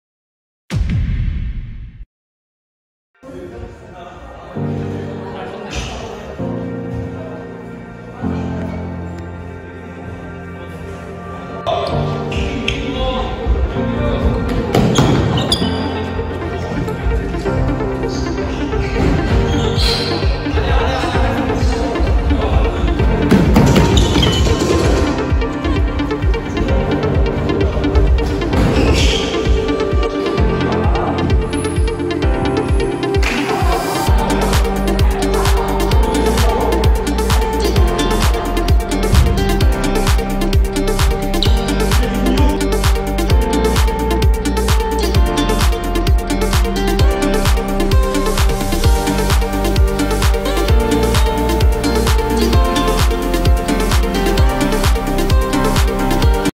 유연성부터 싯업까지 전부 만점!!!ㅣ4월모의테스트 공동3위 건대부고 나선우 [스왓TV]
4월모의테스트 공동3위 건대부고 나선우

20m왕복달리기 - 16.56초
배근력 - 153kg
싯업 - 74회
좌전굴 - 31.2cm

lovesanglee0